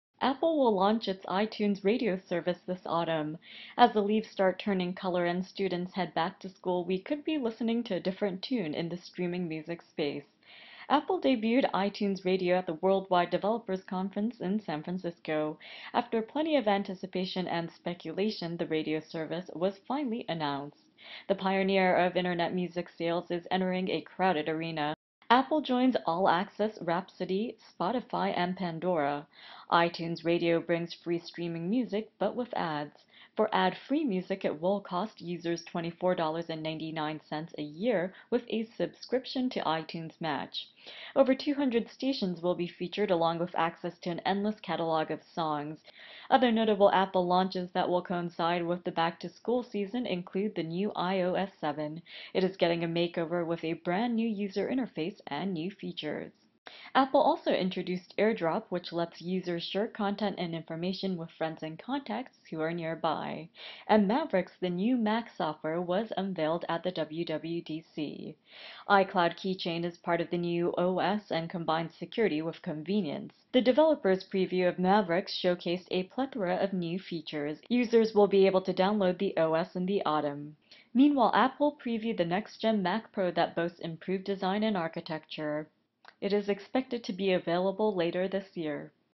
Apple ensures fruitful back-to-school and holiday season
06/10/13 Apple unveiled juicy software and hardware plans at the Worldwide Developers Conference keynote.